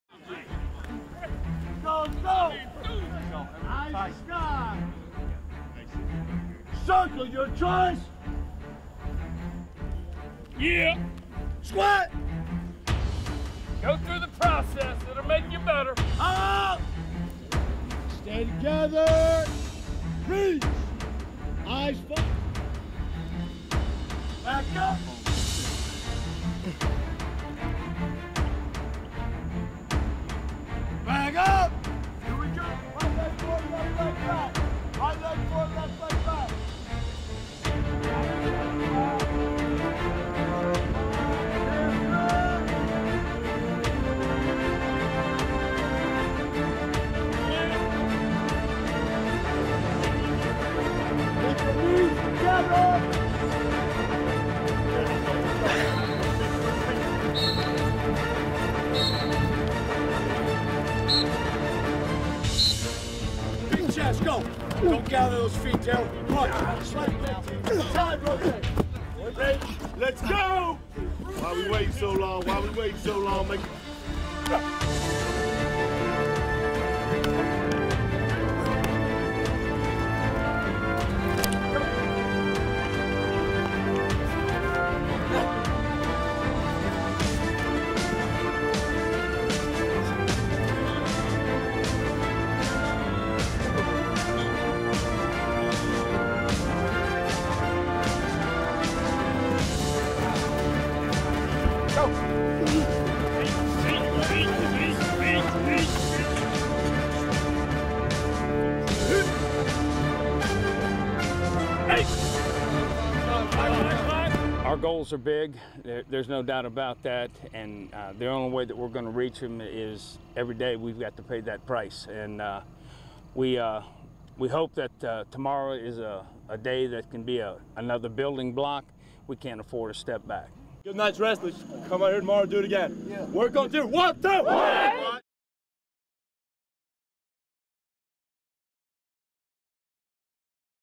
First Football Practice 2014
The Wofford Football team gets 2014 underway with the first practice of the season.